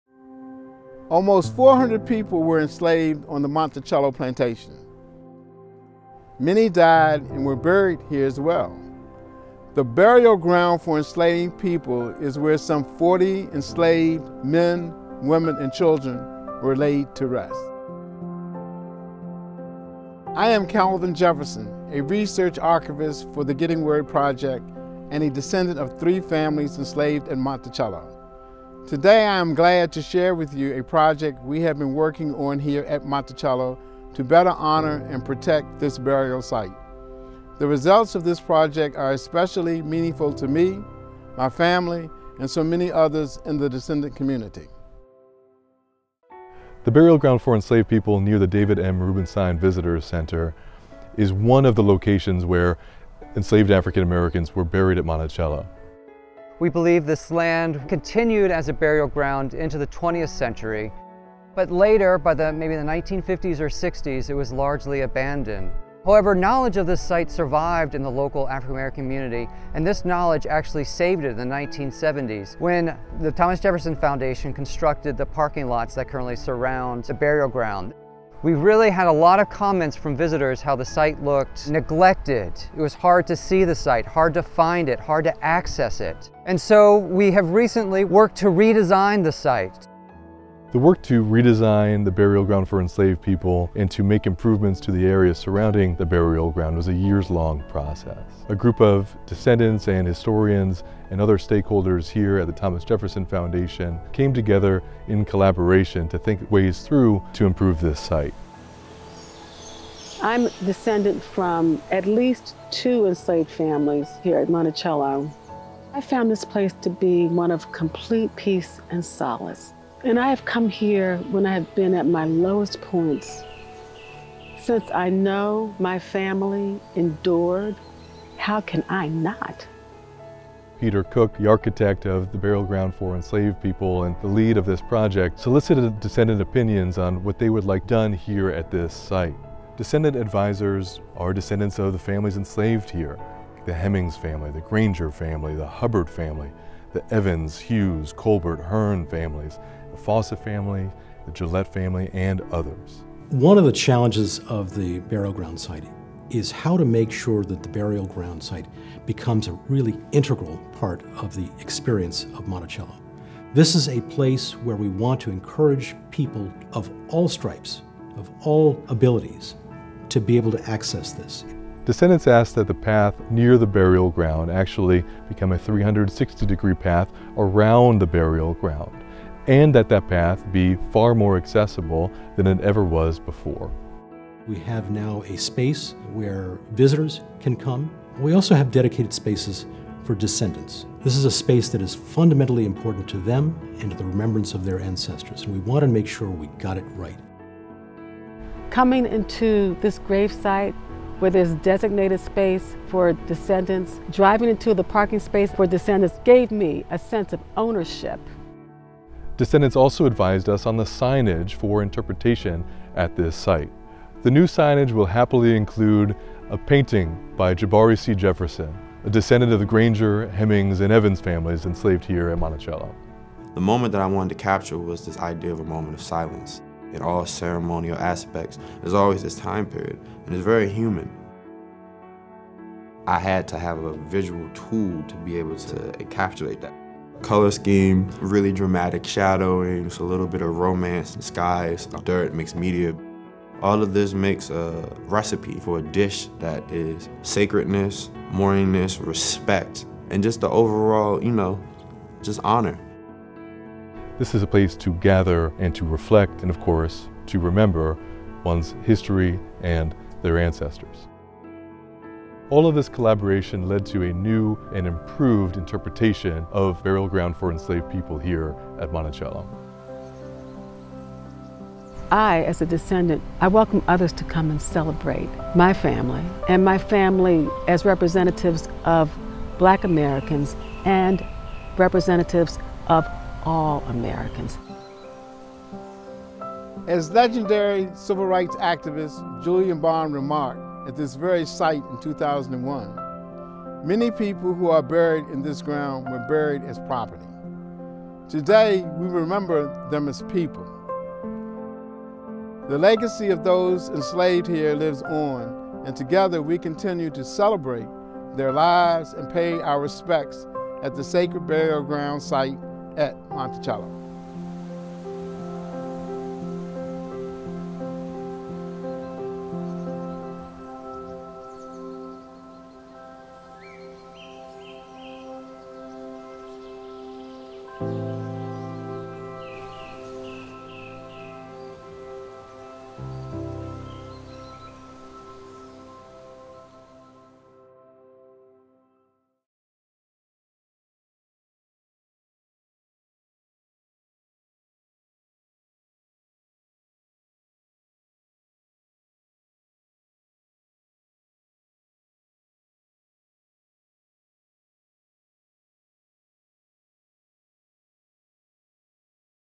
Burial Ground for Enslaved People at Monticello
The Burial Ground for Enslaved People affords an opportunity for honoring and reflecting on the over 400 enslaved people who lived and labored at Monticello during Thomas Jefferson's lifetime. This burial ground, which holds over 40 graves and was likely used beginning in the eighteenth century, was dedicated in 2001 in a ceremony featuring remarks from NAACP President Julian Bond.

Led by guidance of the Getting Word Advisory Committee, a group of descendants representing the interests of multiple families enslaved on this plantation, Monticello embarked on a new initiative to better honor and protect the solemnity of this site in 2018. Completed in 2022, this project introduced more accessible pathways, new plantings, dedicated parking for descendants, additional seating, and new signage.